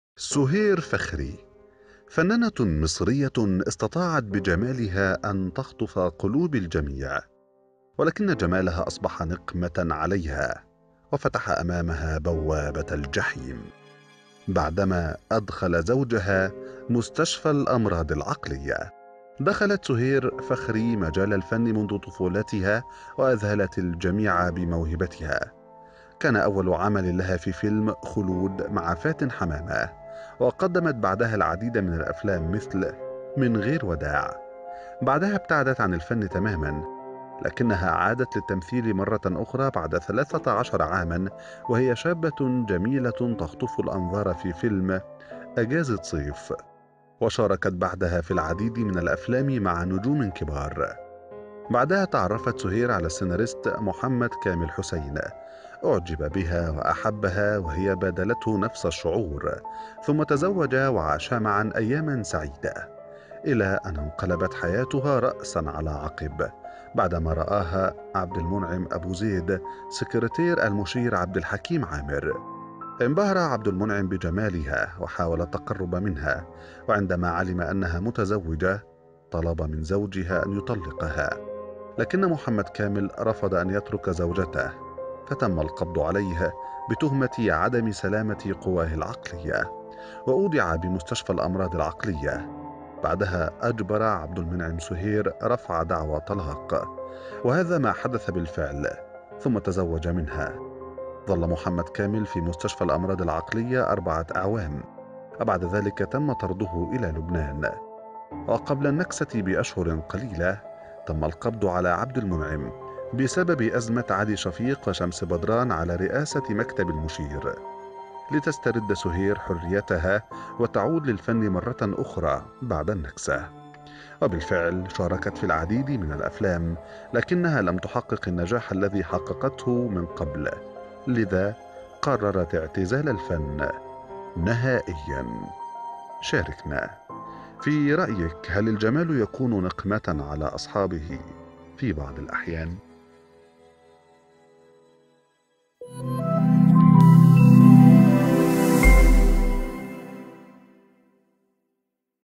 الفنانة المصرية التي تسبب جمالها في دخول زوجها مستشفى الأمراض العقلية "تعرف عليها"
الفنانة المصرية التي تسبب جمالها في دخول زوجها مستشفى الأمراض العقلية "تعرف عليها" في هذا الفيديو
سهير_فخري".. فنانة_مصرية استطاعت بجمالها أن تخطف قلوب الجميع ولكن جمالها أصبح نقمة عليها وفتح أمامها بوابة الجحيم بعدما أدخل زوجها مستشفى الأمراض# العقلية.
دخلت "سهير فخري" مجال الفن منذ طفولتها وأذهلت الجميع بموهبتها كان أول عمل لها فى فيلم "خلود" مع #"فاتن حمامة" وقدمت
بعدها العديد من# الأفلام مثل "من غير وداع" بعدها ابتعدت عن الفن تمامًا، لكنها عادت للتمثيل مرة أخرى بعد 13 عامًا وهي شابة جميلة
تخطف# الأنظار في فيلم "إجازة صيف" وشاركت بعدها في العديد من# الأفلام من نجوم # كبار، بعدها تعرفت "سهير" على
"محمد كامل حسين" وأعجب بها وأحبها وهي بادلته نفس الشعور ثم تزوجا وعاشا معًا أيامًا سعيدة.
إلى أن انقلبت حياتها رأسا على عقب بعدما رآها "عبدالمنعم أبو زيد" سكرتير المشير "عبدالحكيم عامر"، انبهر "عبدالمنعم" بجمالها
وحاول التقرب منها وعندما علم أنها# متزوجة طلب من زوجها أن يطلقها لكن "محمد كامل" رفض أن يترك زوجته فتم القبض عليه
بتهمة عدم سلامة قواه العقلية وأودع بمستشفى الأمراض العقلية، بعدها أجبر "عبدالمنعم" "سهير" رفع دعوى طلاق وهذا ما حدث بالفعل ثم تزوج منها.
ظل "محمد كامل" في مستشفى الأمراض# العقلية 4 أعوام وبعد ذلك تم طرده إلى لبنان وقبل النكسة بأشهر قليلة تم القبض على "عبدالمنعم"
بسبب أزمة "على# شفيق وشمس بدران" على رئاسة مكتب# المشير لتسترد "سهير" حريتها وتعود# للفن مرة أخرى بعد# النكسة
وبالفعل شاركت في العديد من الأفلام  لكنها لم تحقق النجاح الذي حققته من قبل لذا قررت# اعتزال الفن نهائيًا.
شاركنا في رأيك هل الجمال يكون نقمة على أصحابه في بعض الأحيان؟